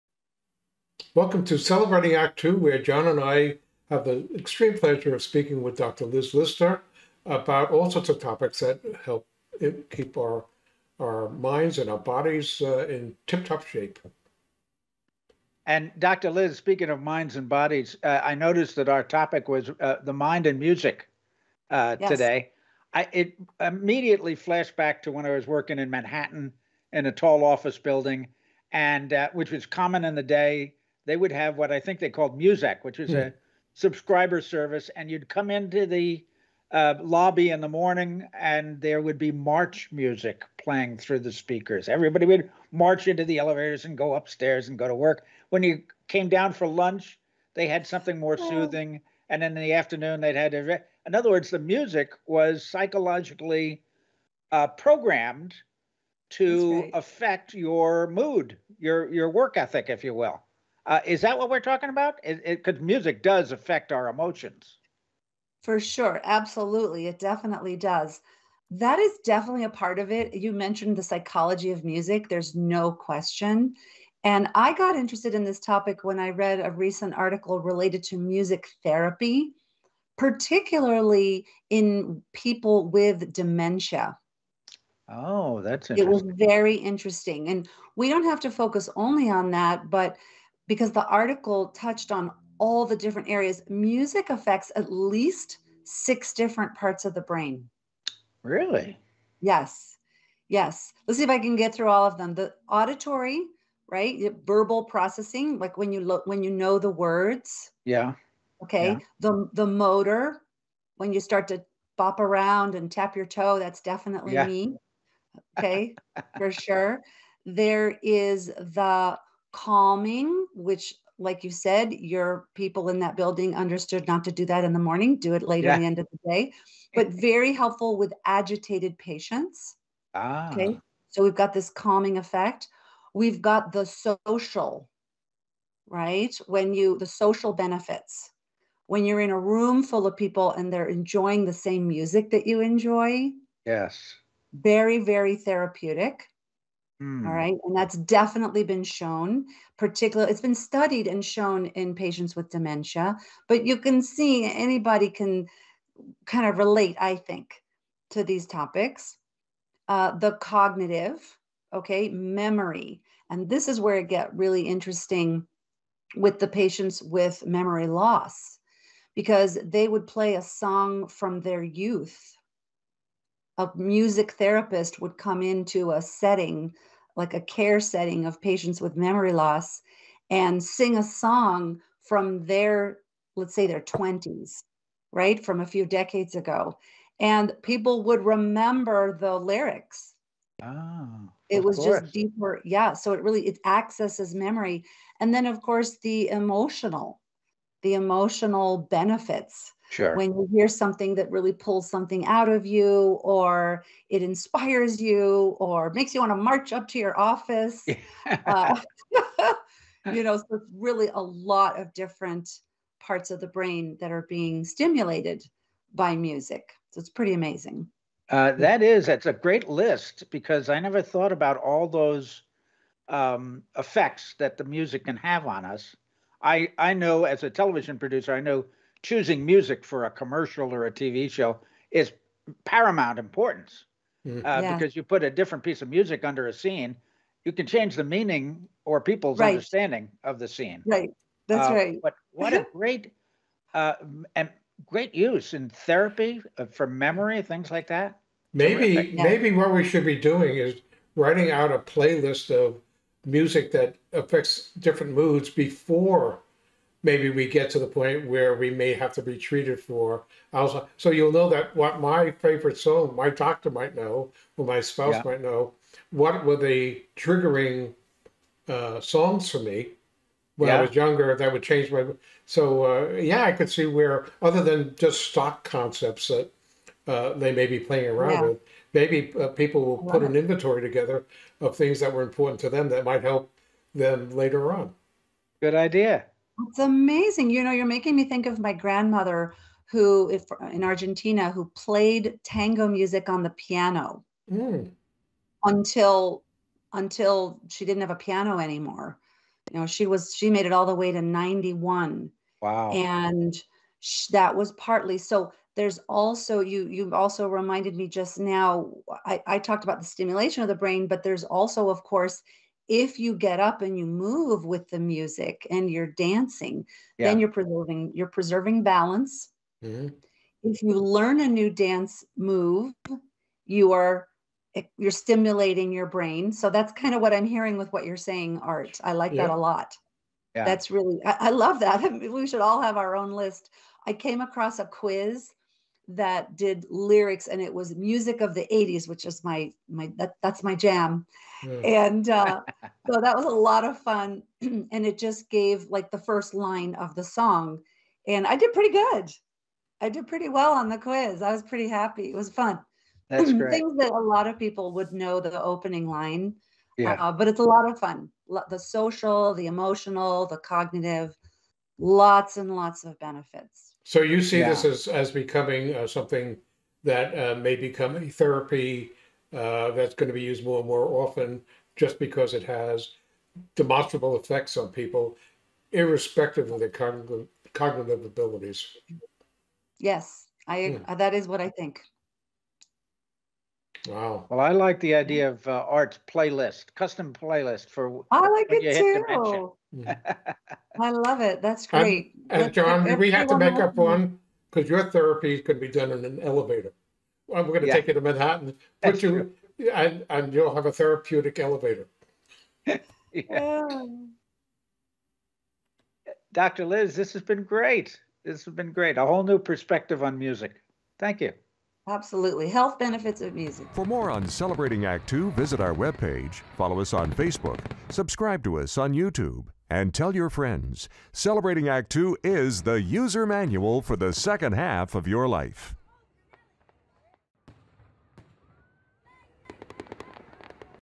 Dr Liz, MD: Music And Your Mind
We've all heard the saying, "Music soothes the savage beast," Dr Liz, MD explains exactly why that is true--in many more ways that you would have thought!

And shop online for Dr Liz' recommended Pharmaceutical Grade Vitamins and Supplements.